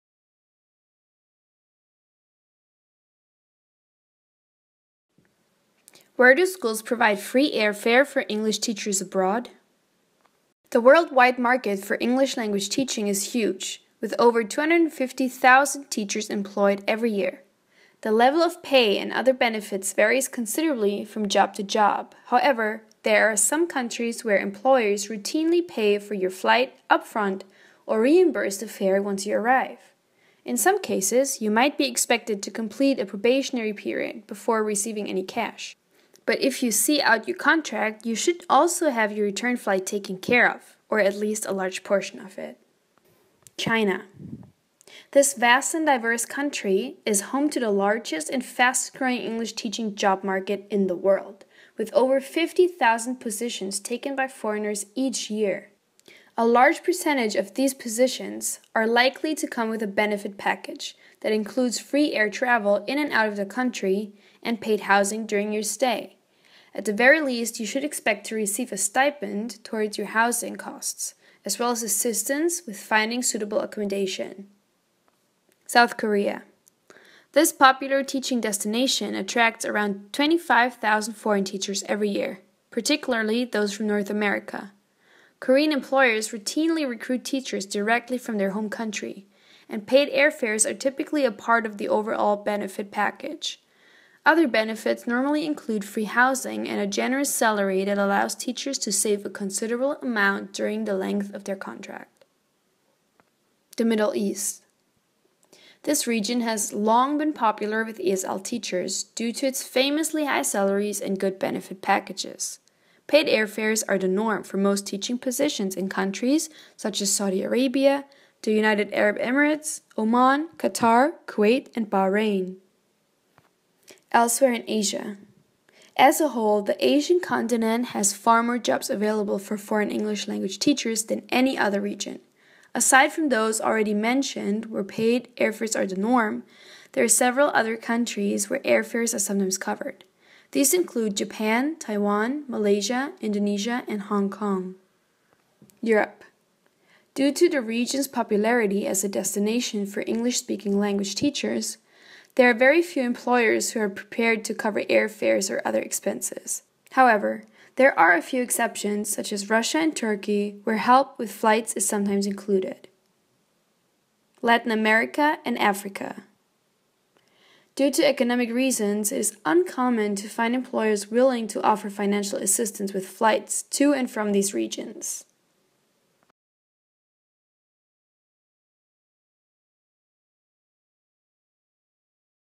ITTT FAQs - Where do schools provide free airfare for English teachers abroad?
The worldwide TEFL/TESOL market is huge with over 250,000 teachers employed each year. The pay and benefits vary from job to job but there are several countries where employers regularly pay for your flight either in advance or after you have arrived. In some cases you may have to complete a probationary period before being reimbursed.

China is the largest market for English language teachers in the world with over 50,000 teachers placed each year. The majority of these positions are likely to include a benefits package that includes return airfares and paid accommodation. At the least you should expect a small stipend towards accommodation and assistance with finding suitable housing.

Other countries in Asia that may offer paid airfares include Japan, Hong Kong, Taiwan, Malaysia and Indonesia.

South Korea is popular destination that attracts 25,000 foreign TEFL//TESOL teacher per year, mostly from North America. Korean employers generally hire teachers directly from their home country and paid airfares are typically part of the overall package. Other benefits include free accommodation and a high salary.

The Middle East is a popular draw for ESL teachers due to the high salaries and generous benefits packages offered by employers. Paid airfares are normal for Saudi Arabia, the UAE, Oman, Qatar, Kuwait and Bahrain.

The popularity of Europe as a destination for English teachers means that there are very few employers who will offer benefits such as paid airfares. However, there are a few exceptions including Russia and Turkey where some employers may offer help towards airfares.

Due to economic reasons it is rare for employers in Africa and Latin America to offer paid airfares and other financial benefits.